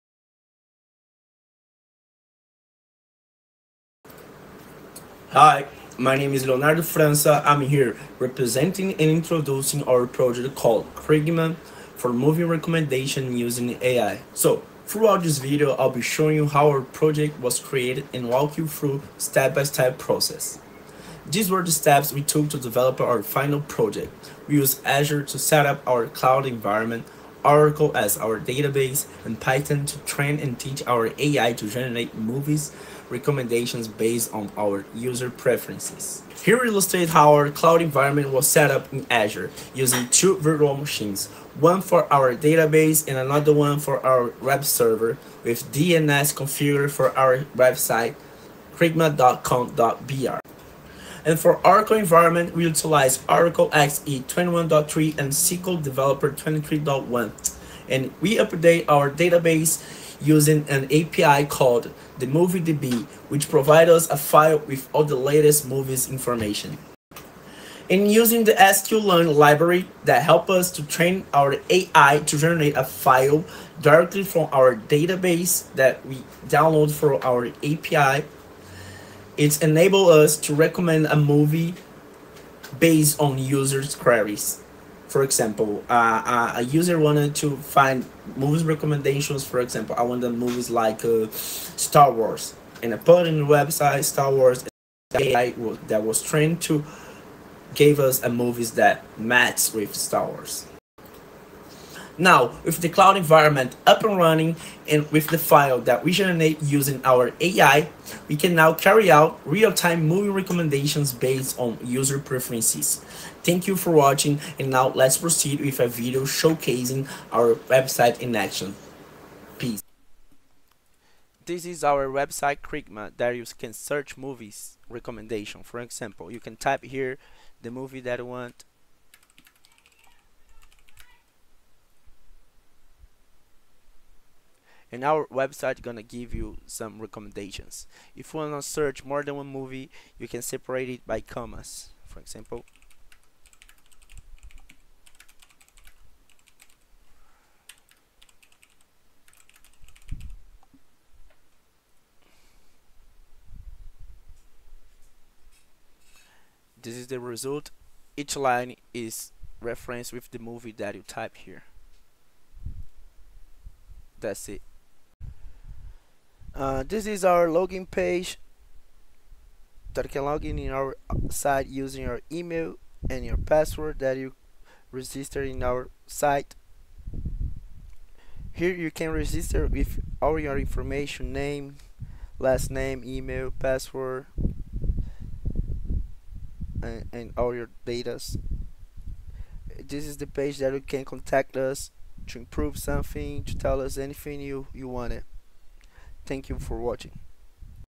CRIGMA
The “Crigma” project is a film recommendation system based on artificial intelligence.

O projeto “Crigma” se trata de um sistema de recomendação de filmes baseada em inteligência artificial.

PRESENTER:
Leonardo França

TEAM:
Diogo Alves Lima
Eduardo Souza
Gabriel Sant'anna Ven Ncio
Hueini David De Paula Dunda
Icaro Moreira
Ingryd Beatriz Silva Rosa
Julio Cezar Maciel Alves
Kevin Alves Do Nascimento
Leonardo França Fontinele De Almeida
Thiago Fabri De Oliveira

CREDTS:
Edson M. De Souza